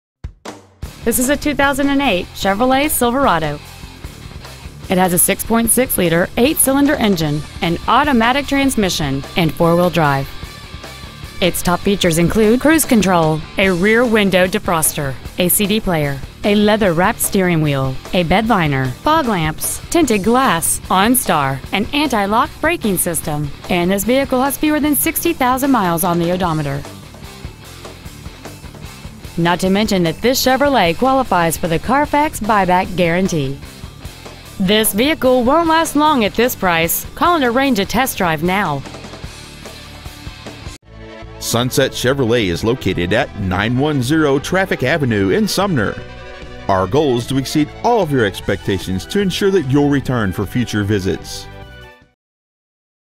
2008 Chevrolet Silverado Sumner WA
Year : 2008
 Make : Chevrolet
 Model : Silverado
 Engine : 6.6L V8 DPI
 Trans . : Automatic
 Exterior : Summit White
 Miles : 59,388
 Interior : Black
 Stock : P31065

 2008 Chevrolet Silverado Sumner WA